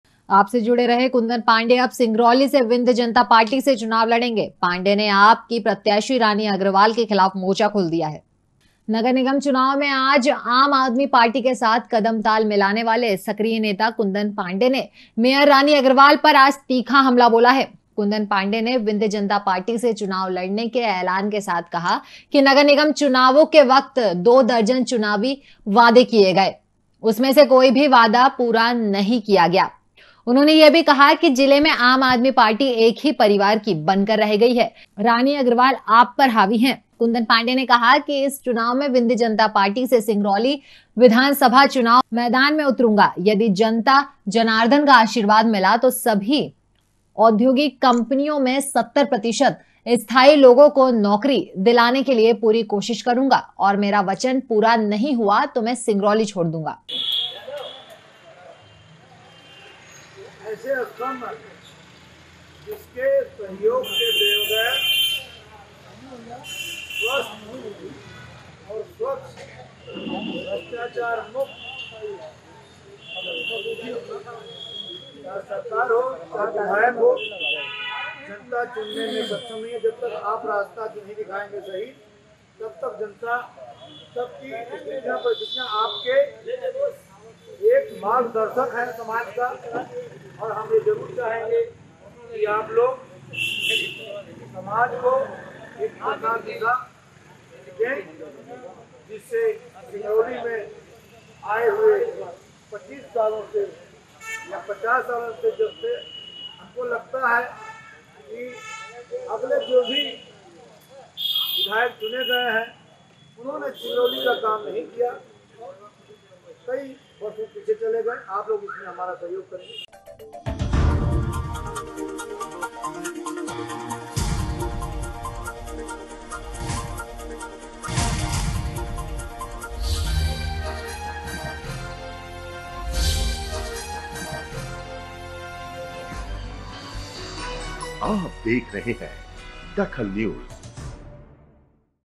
DAKHAL NEWS विंध्य जनता पार्टी से लड़ेंगे कुंदन पांडेय
विंध्य जनता पार्टी से लड़ेंगे कुंदन पांडेय
पांडेय ने रानी अग्रवाल के खिलाफ मोर्चा खोला